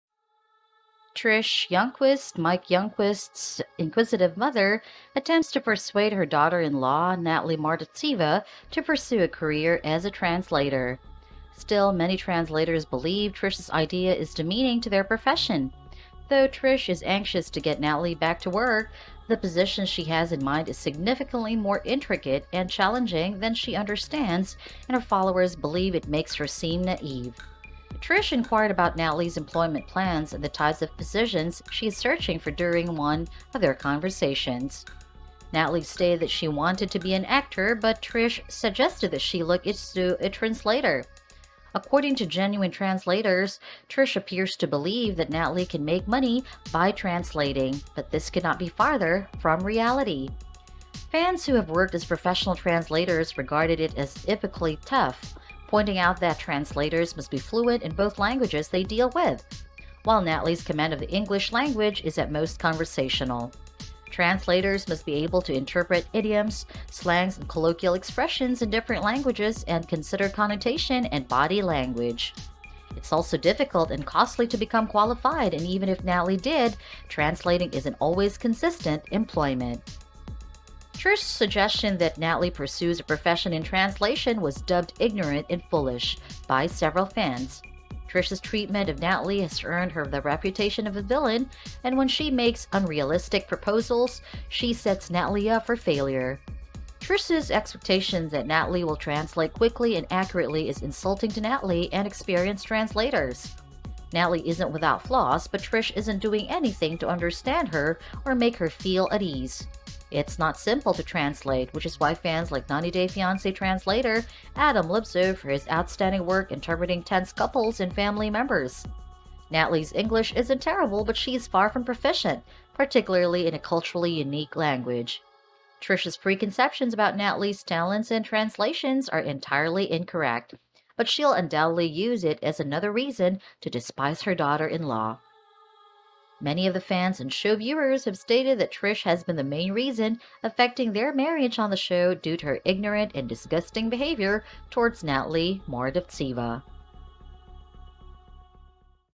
90 Day Fiancé Mike Youngquist Nosy Mom Trish Persuades Natalie Career As Translator
A former character of the reality television show, 90-Day Fiance, Trish Youngquist, mother of Mike Youngquist has been a center of criticisms among raged fans due to her unrealistic perception about Natalie's job.